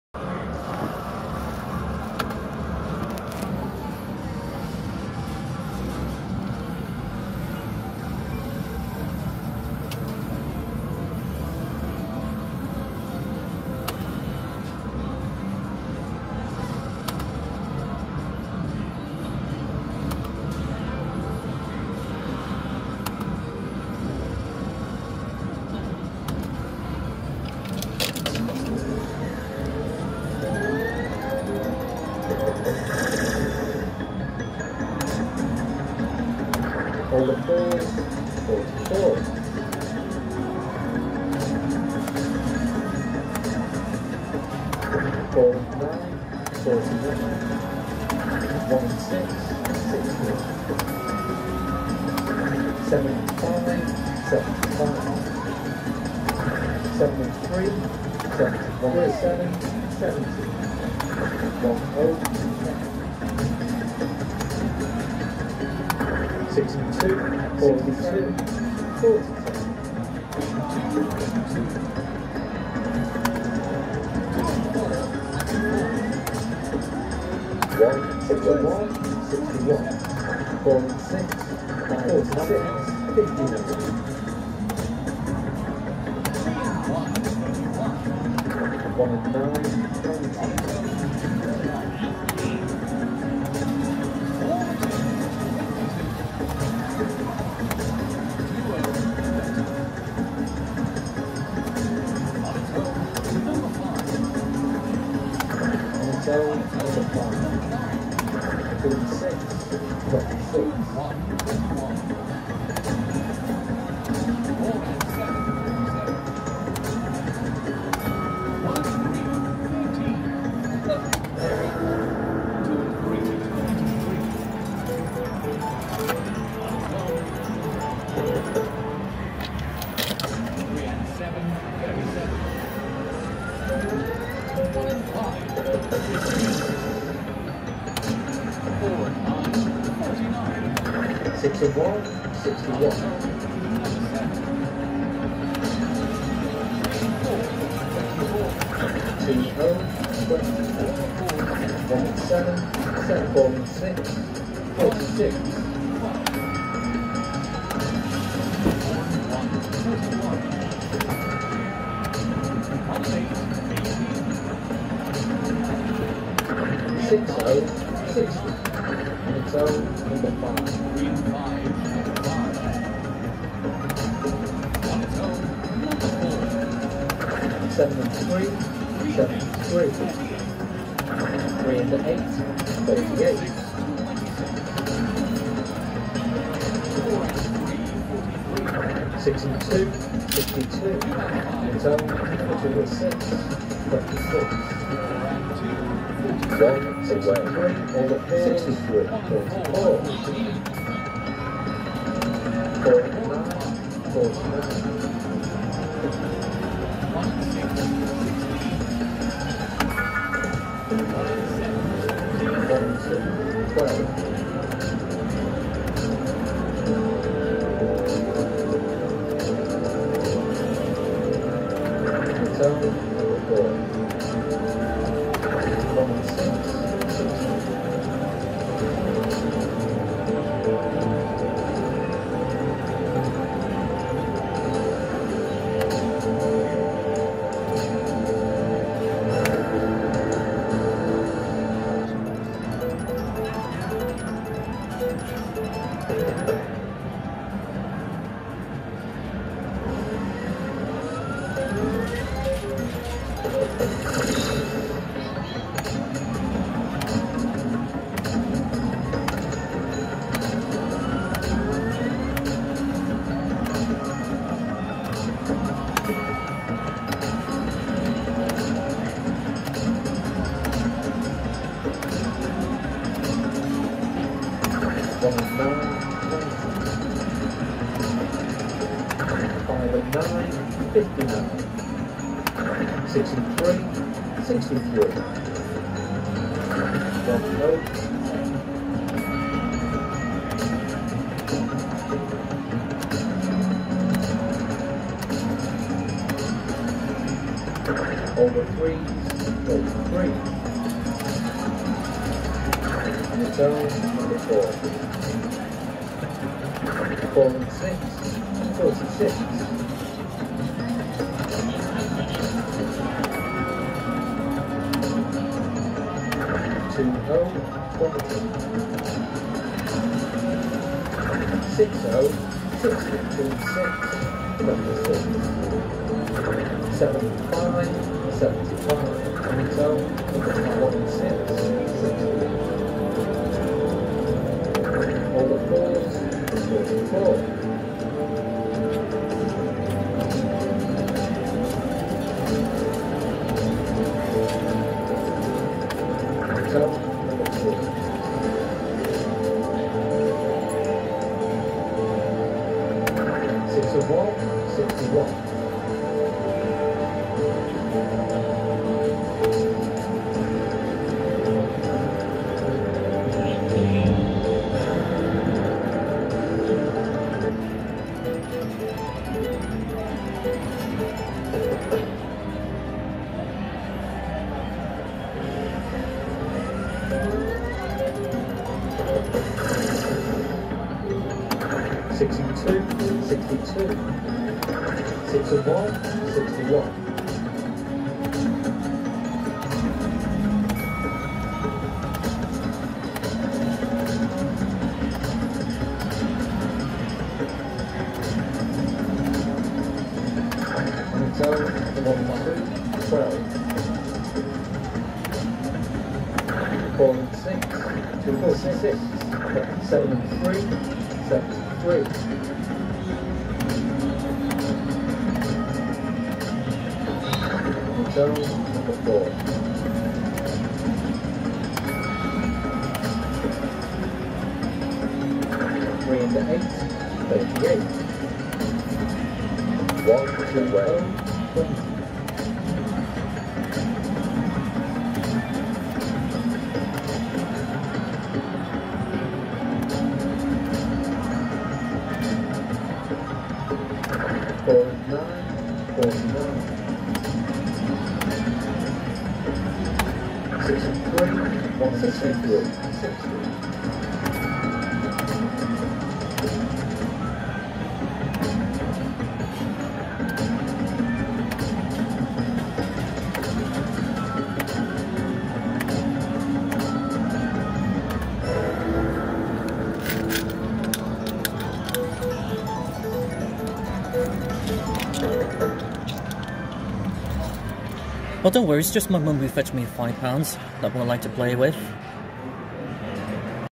Skill Ball Bingo (Zingy) at The Mirage in Mablethorpe
This video is recorded from 13th June 2024.

When you collect all letters to spell the word Zingy, you can get to the jackpot game to win £50 cash, then you got free play when the game is over. Be careful: If you accept the skull, 🟠☠️ you will lose your letters you've collected. The 🔵? ball contains a bonus time which means you can spend more time to play this game.

Remember to match your bingo numbers by pressing red reject button and green accept button. When you look your eyes on bingo numbers: If the number is correct, press accept button. 🟢  If the number is wrong, press reject button. 🔴 If you want to change the bingo cards, press the yellow button. 🟡

Win lines:
No letters - £2
1 letter - £3
2 letters - £5
3 letters - £10
4 letters - £20
5 letters - £50